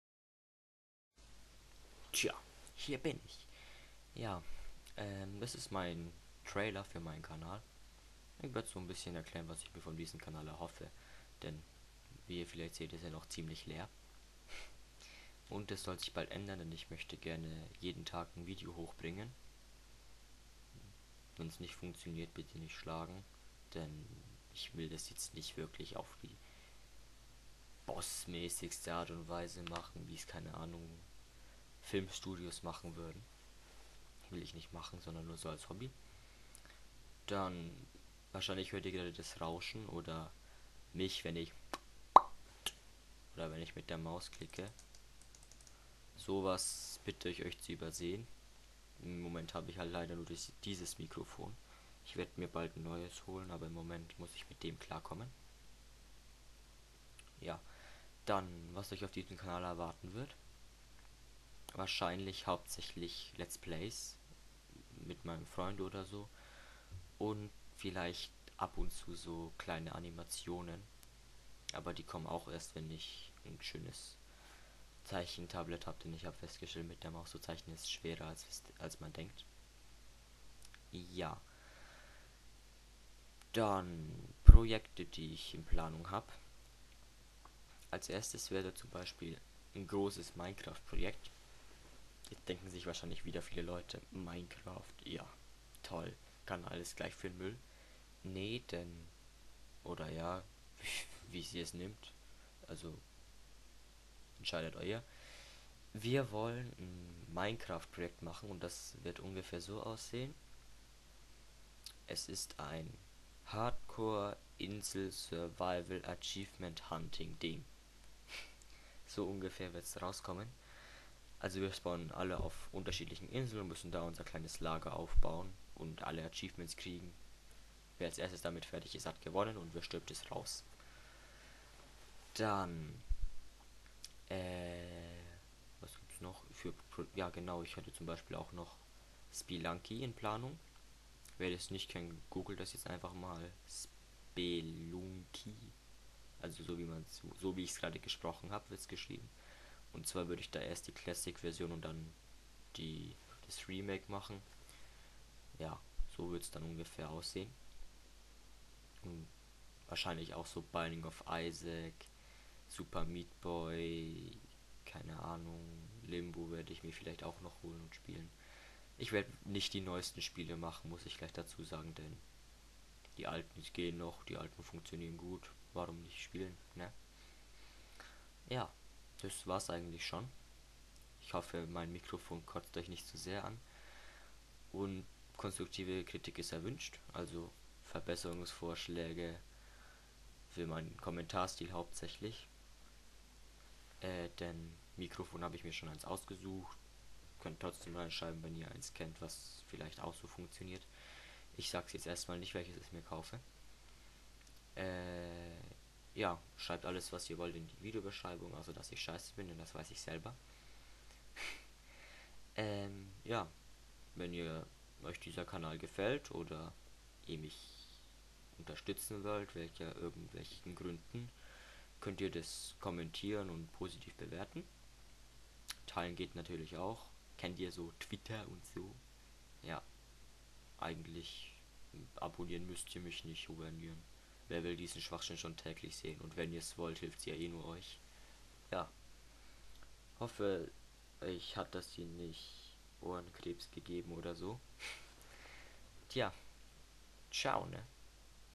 Carlibrauns Trailer (schlechter Sound/schlechte Bilder)
Erstes Video von mir.
Ich erzähle was ich mir von diesem Kanal Erwarte.
Hoffe die schlechte Qualität stört nicht zuviel... :(